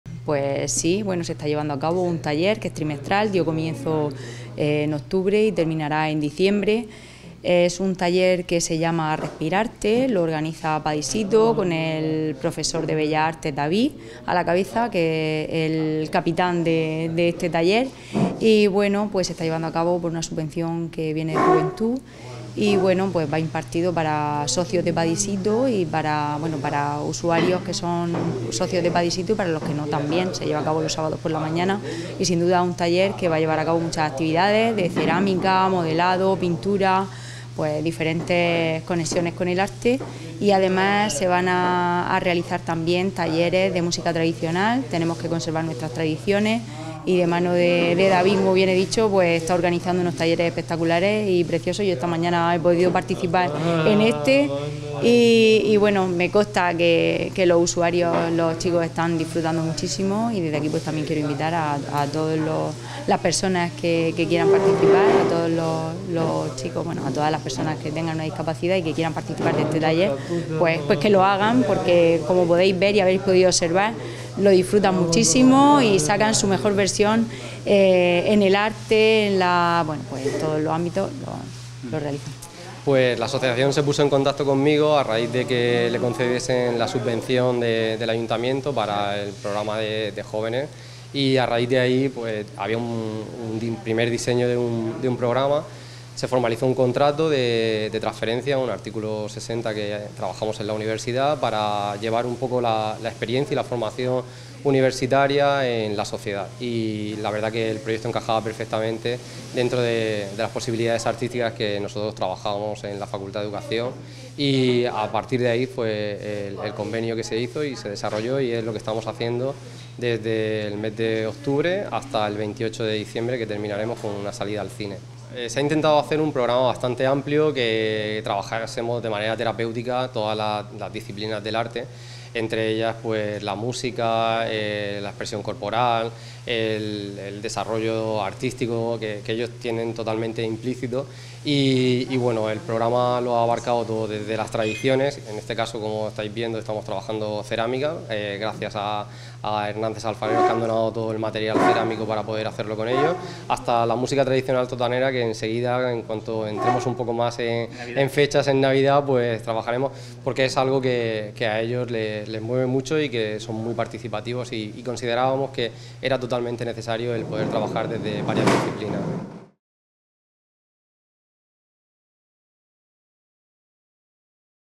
Visitamos el programa de dinamización para jóvenes con discapacidad intelectual, “Respi-Arte”
Asistimos al programa trimestral de dinamización para jóvenes con discapacidad intelectual, “Respi-Arte”, que la Asociación PADISITO, está realizando en virtud de la subvención concedida por la Concejalía de Juventud.